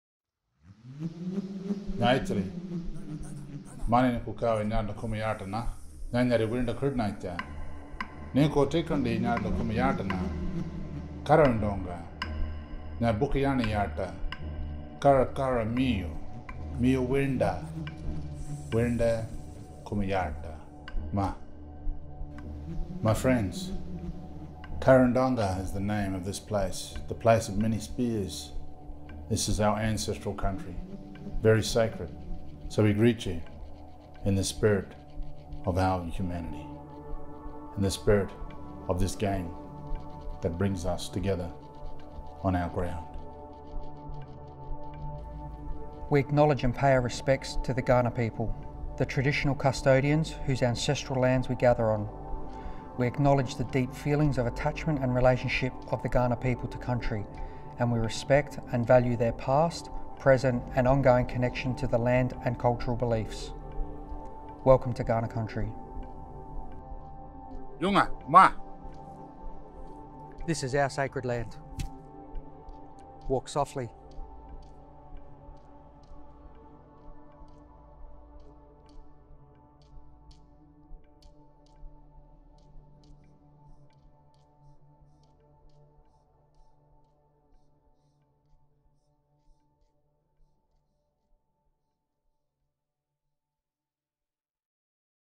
Welcome to Country
Adelaide United's Welcome to Country, which is shown at each home game at Coopers Stadium.